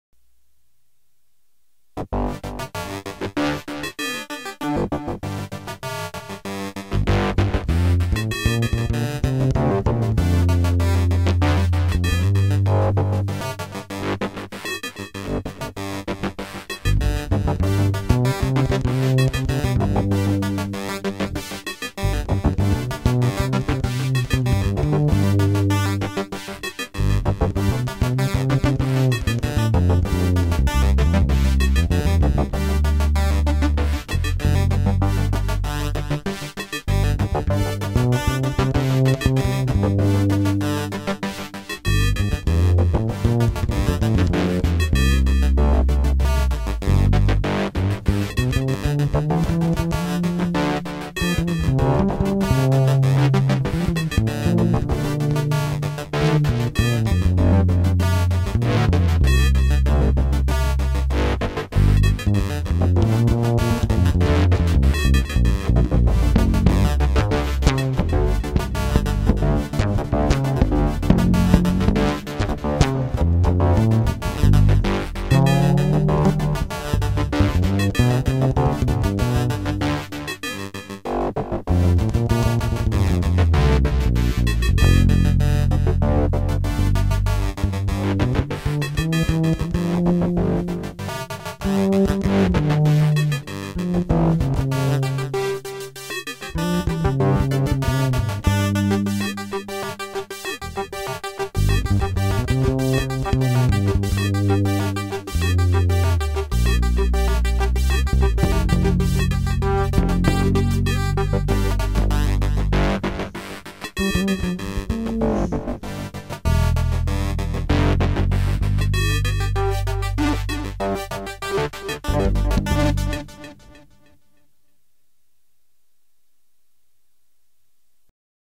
Ashbory Bass with a Korg Kaossilator
A short little improvisation on the Ashbory with the Kaossilator being played with my foot. My chops on the rubber bass are a little bit rusty here, due to the fact that I haven't touched the instrument in over a year.